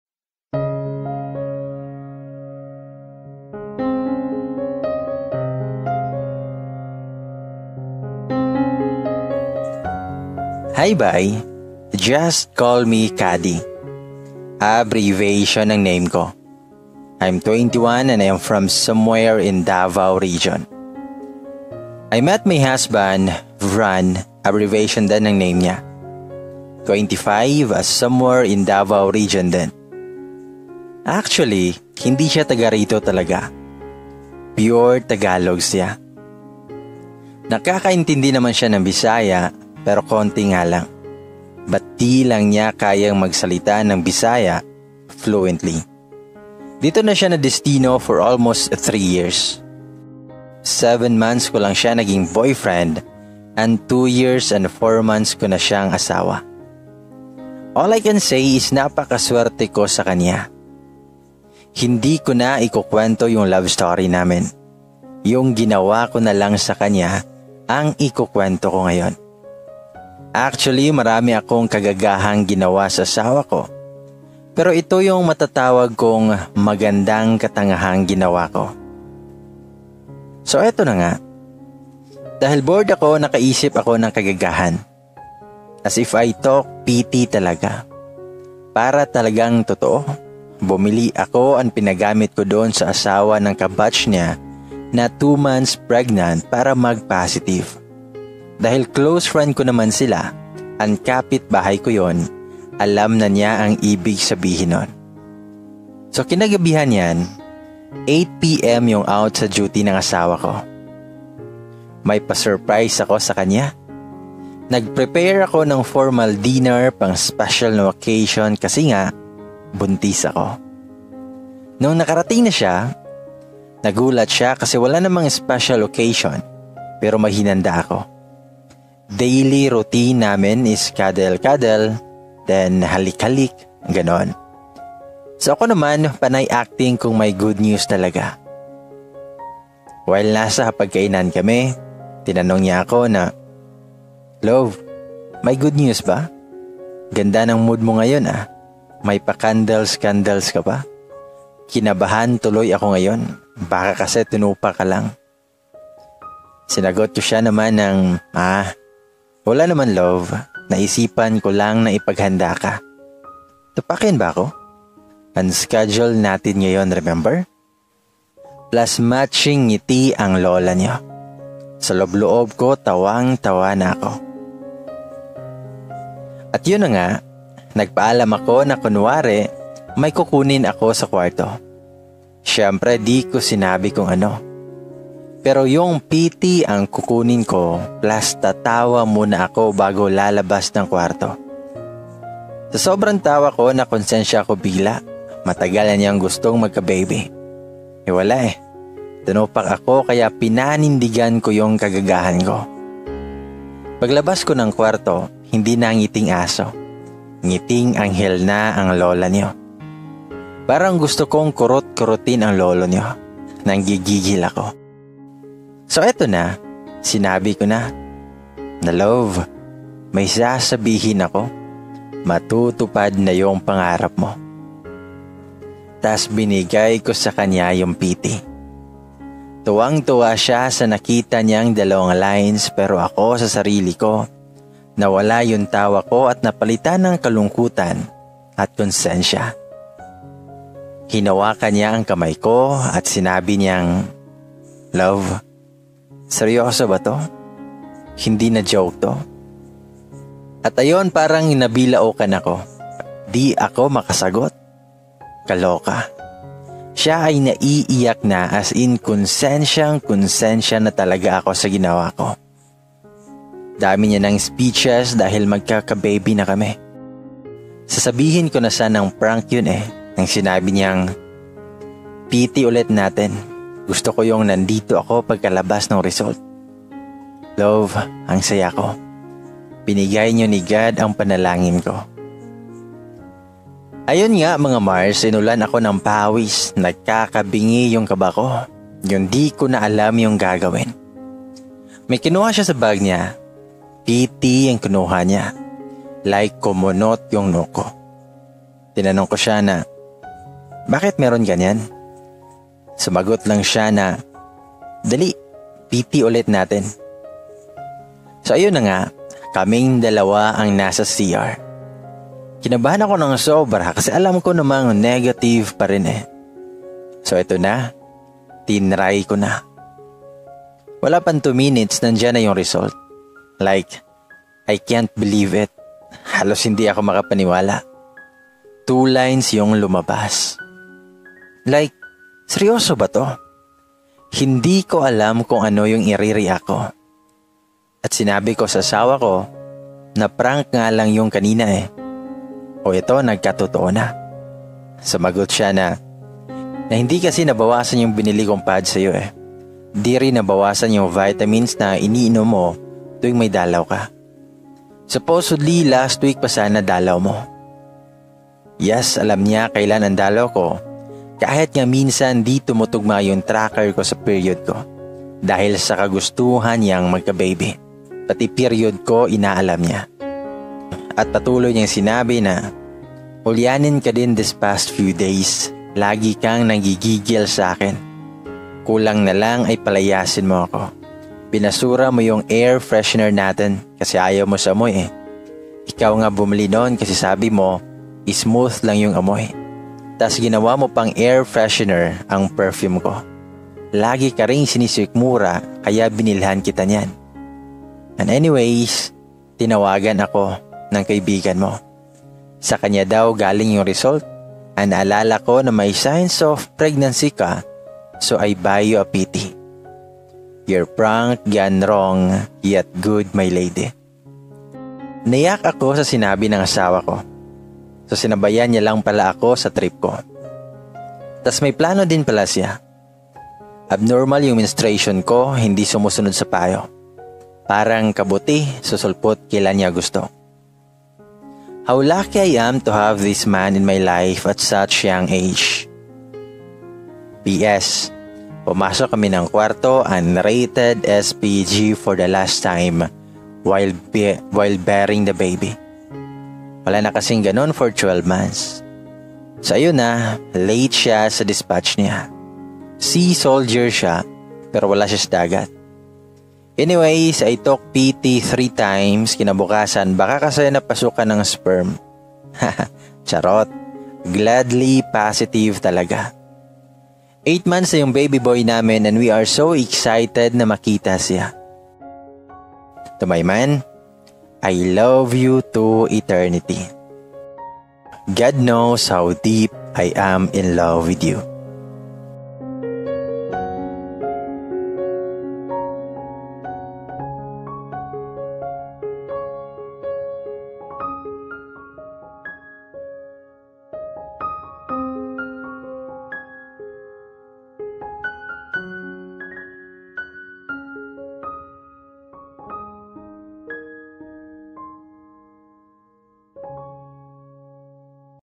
Buntis Prank To My Asawa (Tagalog Confession)| bay gamay reads
bay gamay reads your story, your confession.

Salamat Ladies and Bays!